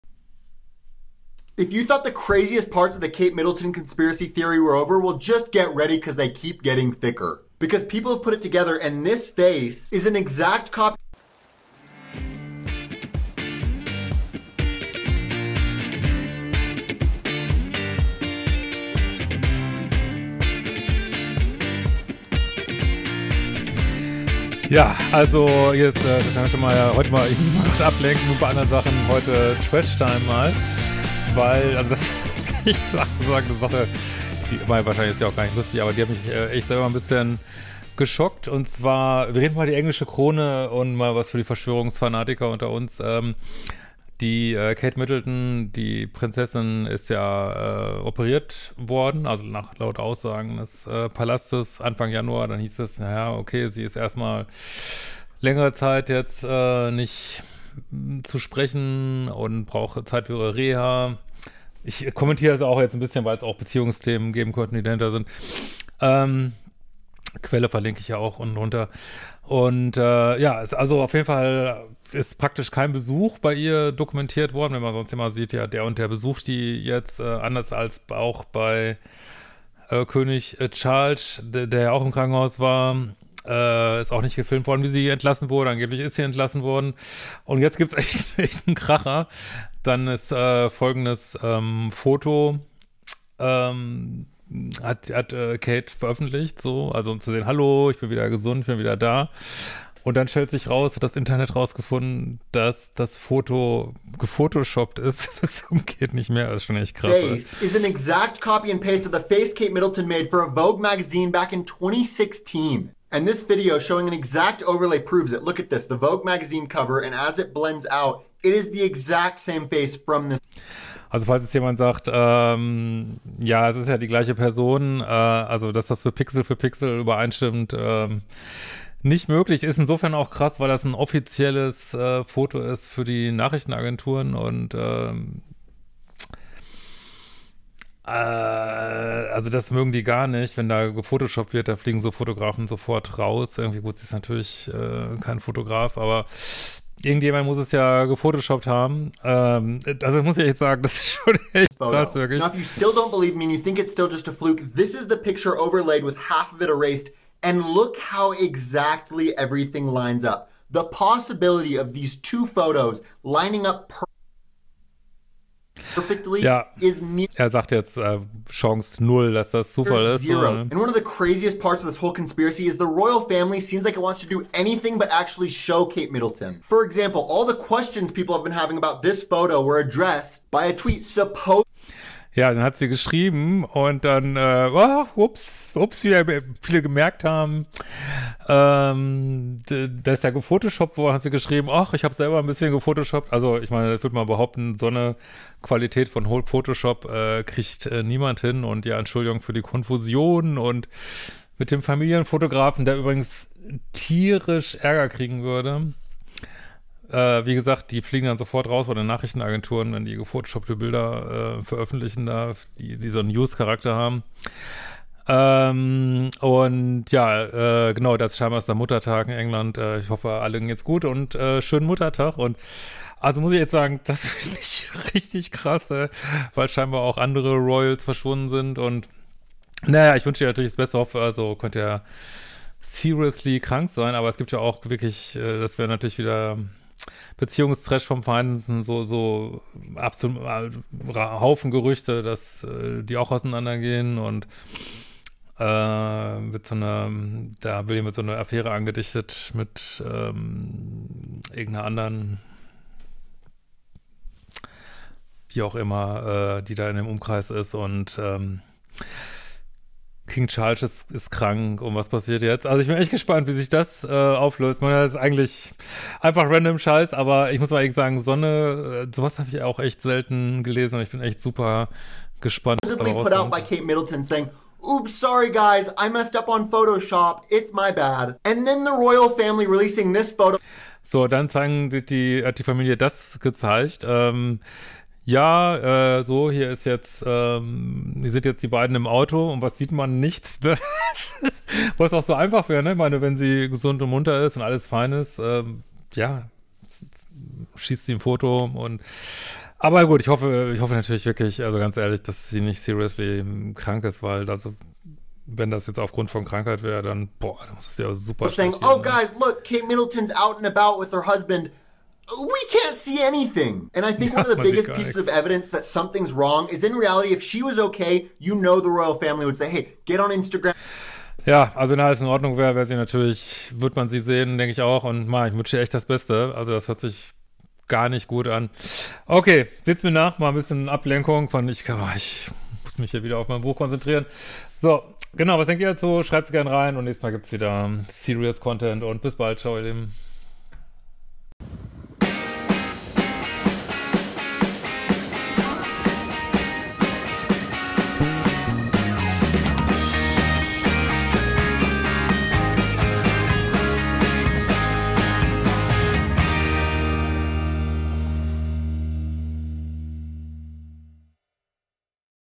Trash time: Wo ist Kate Middleton?
Leute ich muss mich mal ablenken mit etwas anderem, zu viel Home Office gerade. Diese Geschichte um das Verschwinden von Kate hat mich echt gepackt. Was denkt ihr?